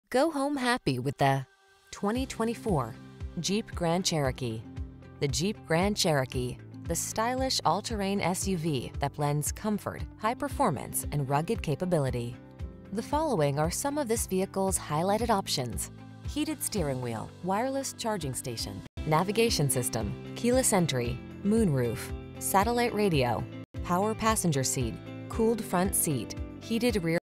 2024 Jeep Grand_Cherokee Overland KS Olathe, Overland Park, Lawrence, Kansas City, Lees Summit
New 2024 Jeep Grand_Cherokee Overland available at Olathe DCJR located in 15500 W 117th Street, Olathe, KS, 66062. Servicing the Olathe, Overland Park, Lawrence, Kansas City, Lees Summit area.

Go home happy with the  2024  JEEP Grand Cherokee. The Jeep Grand Cherokee: The stylish all-terrain SUV that blends comfort, high-performance and rugged capability. The following are some of this vehicles highlighted options: Heated Steering Wheel
, Wireless Charging Station, Navigation System, Keyless Entry, Moonroof, Satellite Radio, Power Passenger Seat, Cooled Front Seat(s), Heated Rear Seat(s), Power Liftgate. Take control of the journey in comfort and style. Get into the Grand Cherokee.